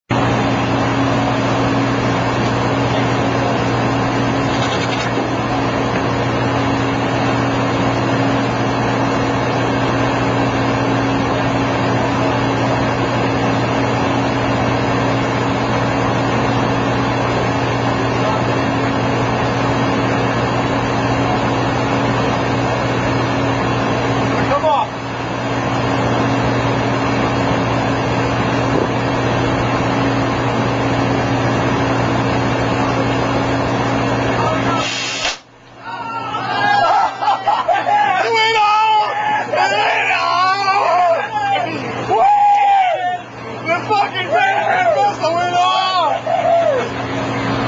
m.r.e missile made by u.s navy deepsea divers
this is a m.r.e missie made from a water bottle! three m.r.e heaters were used in this launch.
no animals were harmed in the making in this video but  if there were some around it probably would have!!hahaha